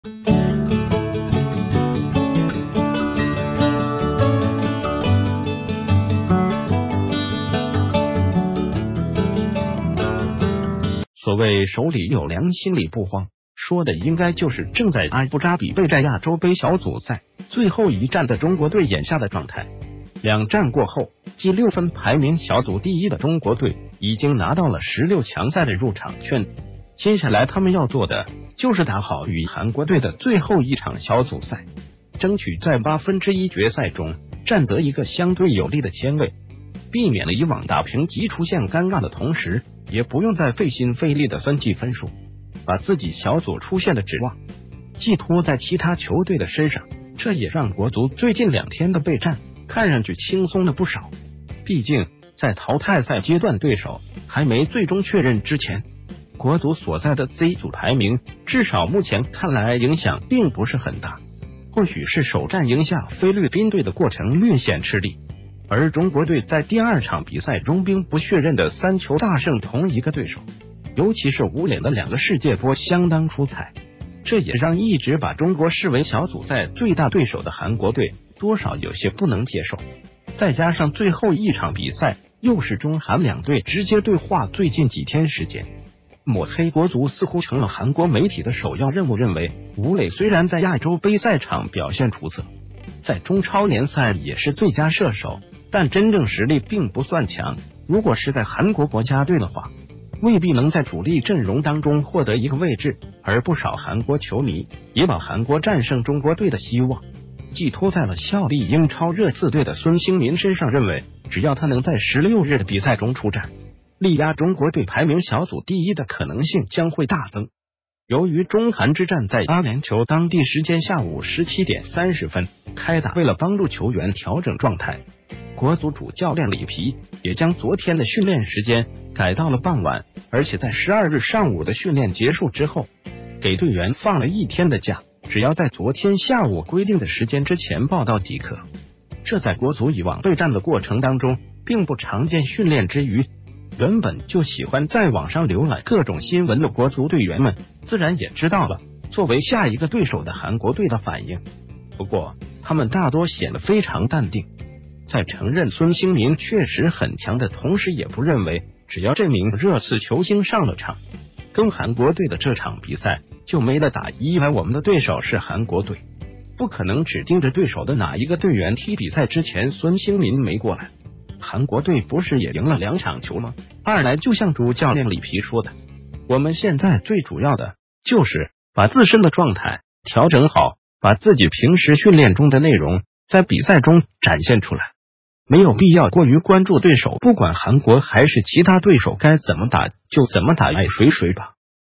里皮罕見給國足放假，孫興慜不是“焦點話題”
谢谢！

    所謂“手里有糧心里不慌”，說的應該就是正在阿布扎比備戰亞洲杯小組賽最后一戰的中國隊眼下的狀態。
    兩戰過后，積六分排名小組第一的中國隊已經拿到了十六強賽的入場券，接下來他們要做的，就是打好與韓國隊的最后一場小組賽，爭取在八分之一決賽中占得一個相對有利的簽位，避免了以往“打平即出線”尷尬的同時，也不用再費心費力地算計分數，把自己小組出線的指望，寄托在其他球隊的身上，這也讓國足最近兩天的備戰看上去輕松了不少，畢竟在淘汰賽階段對手還沒最終確認之前，國足所在的C組排名至少目前看來影響并不是很大。
    或許是首戰贏下菲律賓隊的過程略顯吃力，而中國隊在第二場比賽中兵不血刃地三球大勝同一個對手，尤其是武磊的兩個世界波相當出彩，這也讓一直把中國視為小組賽最大對手的韓國隊多少有些不能接受，再加上最后一場比賽又是中韓兩隊“直接對話”，最近幾天時間，“抹黑”國足似乎成了韓國媒體的首要任務，認為武磊雖然在亞洲杯賽場表現出色，在中超聯賽也是最佳射手，但真正實力并不算強，如果是在韓國國家隊的話，未必能在主力陣容當中獲得一個位置。
    而不少韓國球迷也把韓國戰勝中國隊的希望，寄托在了效力英超熱刺隊的孫興慜身上，認為只要他能在16日的比賽中出戰，力壓中國隊排名小組第一的可能性將會大增。
    由于中韓之戰在阿聯酋當地時間下午17點30分開打，為了幫助球員調整狀態，國足主教練里皮也將昨天的訓練時間改到了傍晚，而且在12日上午的訓練結束之后，給隊員放了一天的假，只要在昨天下午規定的時間之前報到即可，這在國足以往備戰的過程當中并不常見。
    訓練之余，原本就喜歡在網上瀏覽各種新聞的國足隊員們自然也知道了作為下一個對手的韓國隊的反應，不過他們大多顯得非常淡定，在承認孫興慜確實很強的同時，也不認為只要這名熱刺球星上了場，跟韓國隊的這場比賽就沒得打。“一來我們的對手是韓國隊，不可能只盯著對手的哪一個隊員踢比賽，之前孫興慜沒過來，韓國隊不是也贏了兩場球嗎？二來就像主教練里皮說的，我們現在最主要的就是把自身的狀態調整好，把自己平時訓練中的內容在比賽中展現出來，沒有必要過于關注對手，不管韓國還是其他對手，該怎么打就怎么打，愛誰誰吧。”